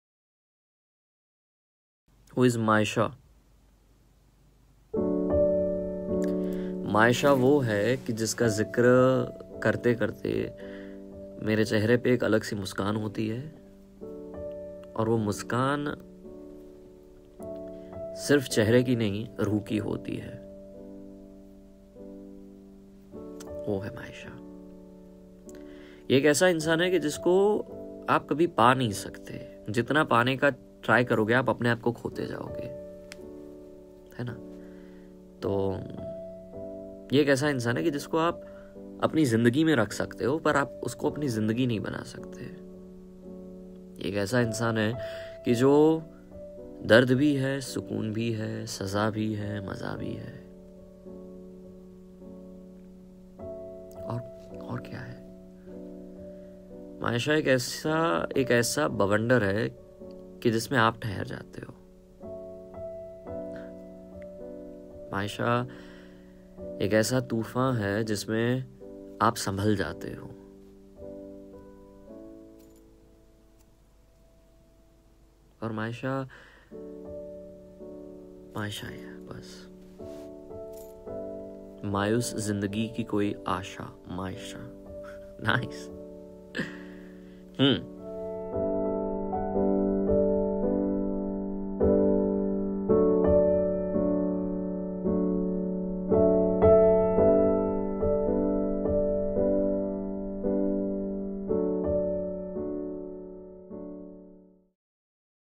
कौन है मायशा - Kuchh Khat Maayesha Ko - RJ Vashishth
While doing FB live someone asked me - kaun hai maayesha..
and this was not the first time that someone had asked me this sawal..
to socha, apne hi andaz me bata dete hain.. kaun hai maayesha..

Matlab, Main bata toh nahi sakta ki kaun hai maayesha
par jata toh sakta hoon ki kaun hai..

my tripod
my camera gear
my editing gear